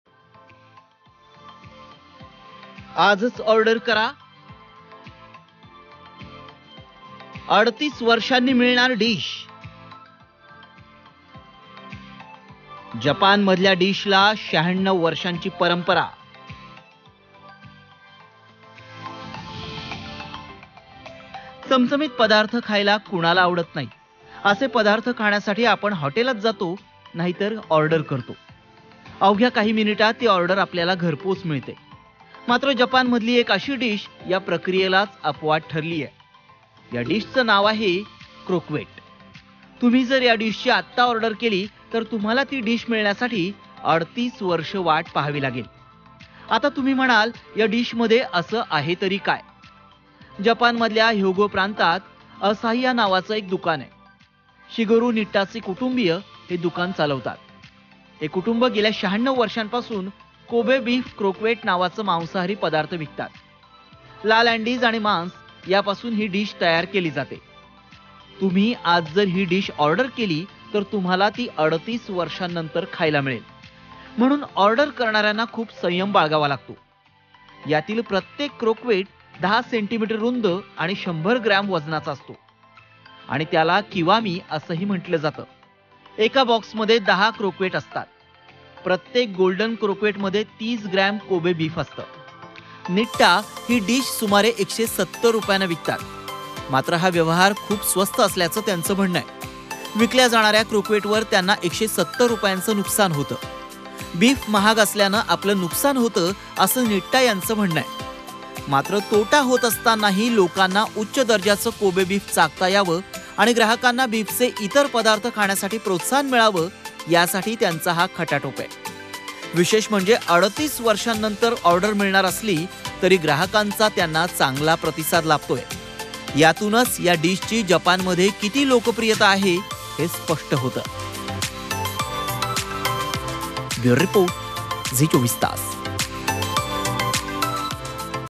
Special Report On Tokyo Famous Dish | आज ऑर्डर केली तर, 38 वर्षांनी मिळणार डिश; पाहा रिपोर्ट
-आज ऑर्डर करा
-38 वर्षांनी मिळणार डिश !
- जपानमधल्या डिशला 96 वर्षांची परंपरा

चमचमीत पदार्थ खायला कुणाला आवडत नाही. असे पदार्थ खाण्यासाठी आपण हॉटेलात जातो नाही तर ऑर्डर करतो. अवघ्या काही मिनिटांत ती ऑर्डर आपल्याला घरपोच मिळते. मात्र जपानमधली एक डिश या प्रक्रियेलाच अपवाद ठरलीय..या डिशचं नाव क्रोक्वेट..तुम्ही जर या डिशची आत्ता ऑर्डर केली तर तुम्हाला ती डिश मिळण्यासाठी 38 वर्षे वाट पाहावी लागेल. आता तुम्ही म्हणाल या डिशमध्ये असं आहे तरी काय? जपानमधल्या ह्योगो प्रांतात असाहिया नावाचं एक दुकान आहे,  शिगेरू निट्टाचे कुटुंब हे दुकान चालवतात.. हे कुटुंब गेल्या 96 वर्षांपासून कोबे बीफ क्रोक्वेट नावाचे मांसाहारी पदार्थ विकतात.  लाल अँडीज आणि मांस यापासून ही डिश तयार केली जाते. तुम्ही  आज जर ही डिश ऑर्डर केली तर तुम्हाला ती 38 वर्षांनंतर खायला मिळेल, म्हणून ऑर्डर करणा-यांना खूप संयम बाळगावा लागतो. यातील प्रत्येक क्रोक्वेट10 सेमी रुंद आणि 100 ग्रॅम वजनाचा असतो आणि त्याला 'किवामी' असंही म्हणतात. एका बॉक्समध्ये 10 क्रोक्वेट असतात. प्रत्येक गोल्डन क्रोक्वेटमध्ये 30 ग्रॅम कोबे बीफ असतं. निट्टा ही डिश सुमारे 170 रुपयांना विकतात, मात्र हा व्यवहार खूप स्वस्त असल्याचं त्यांचं म्हणणं आहे. विकल्या जाणाऱ्या क्रोक्वेटवर त्यांचं 170 रुपयांचं नुकसान होतं, बीफ महाग असल्यानं आपलं नुकसान होतं असं निट्टा यांचं म्हणणं आहे. मात्र तोट होत असतानाही लोकांना उच्च दर्जाचं कोबे बीफ चाखता यावं आणि ग्राहकांना बीफचे इतर पदार्थ खाण्यासाठी प्रोत्साहन मिळावं यासाठी त्यांचा हा खटाटोप आहे. विशेष म्हणजे 38 वर्षानंतर ऑर्डर मिळणार असली तरीही ग्राहकांचा त्यांना चांगला प्रतिसाद लाभतोय. यातच या डिशची जपानमध्ये किती लोकप्रियता आहे हे स्पष्ट होतं.